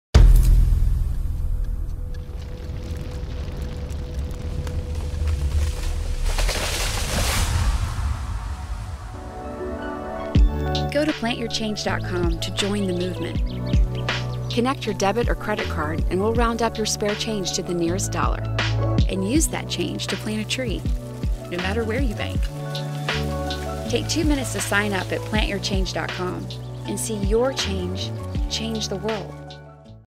Plant Your Change: Use Any Debit or Credit Card to Reforest The Planet
Small actions add up to big change. Plant Your Change lets you fight climate change with every purchase. We make it easy for you to contribute to the solution.